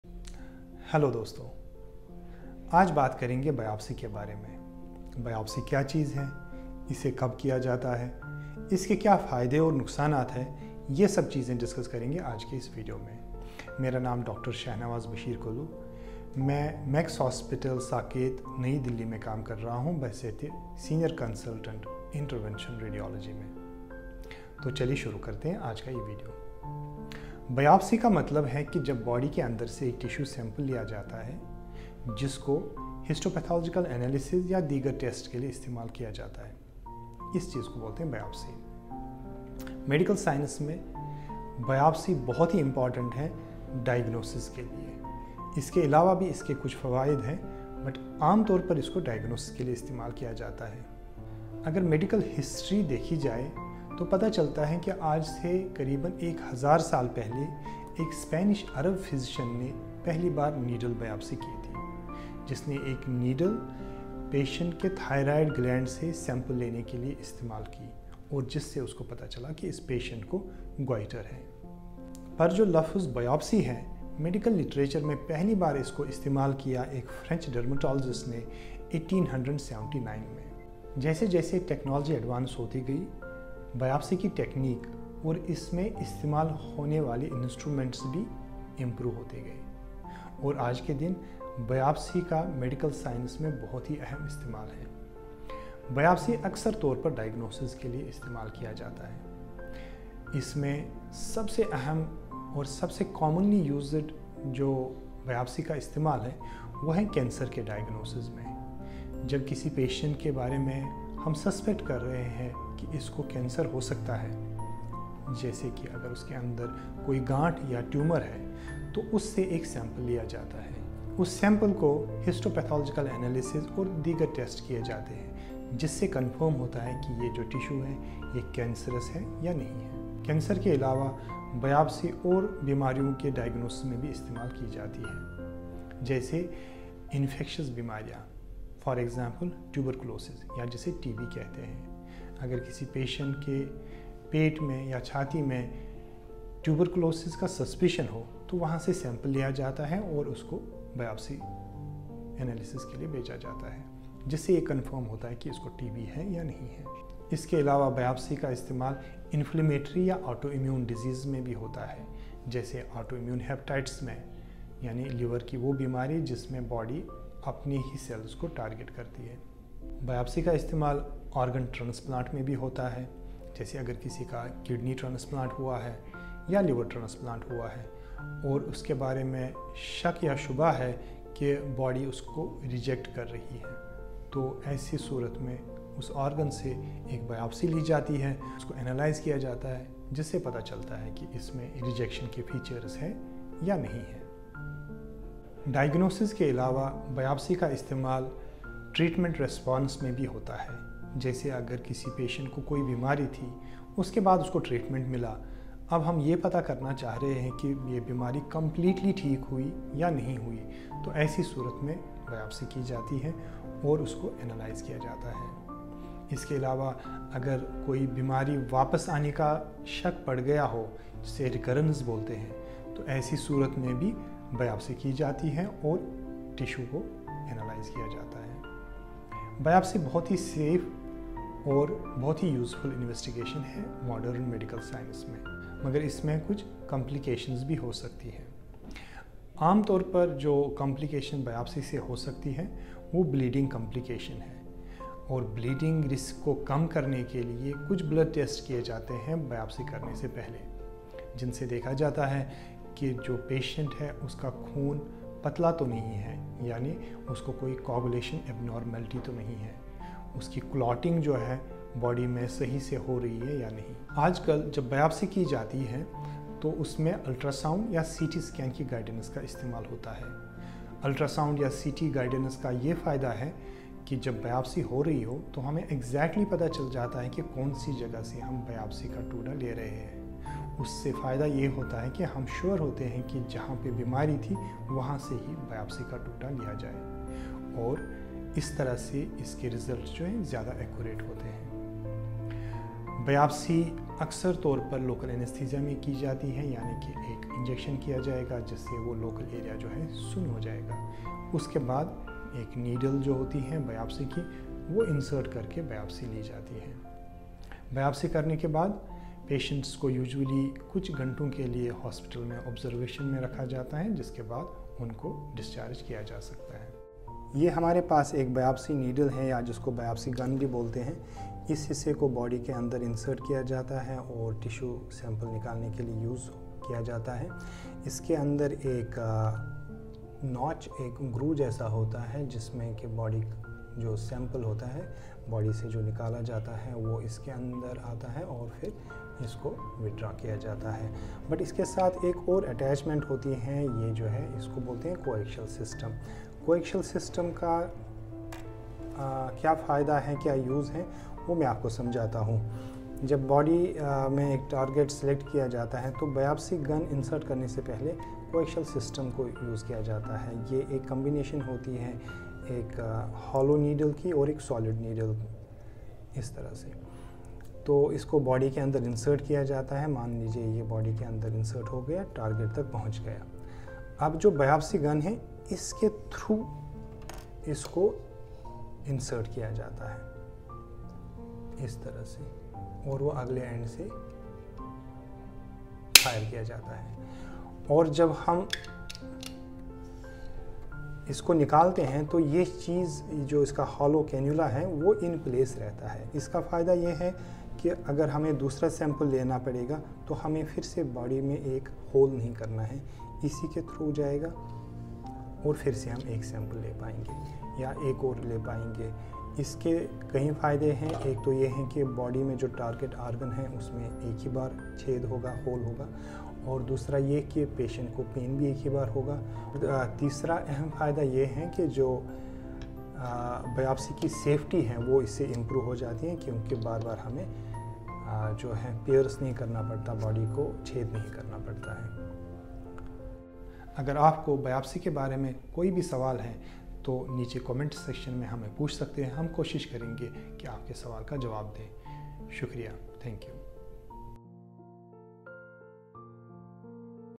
Biopsy: What, When and How? Urdu | Hindi | Dr Shahnawaz B Kaloo | Max Hospital Saket Delhi
Biopsy : Definition, Uses, and Precautions

Please drop your questions in the comments section.

You can also WhatsApp us your medical records at: +917835O56123.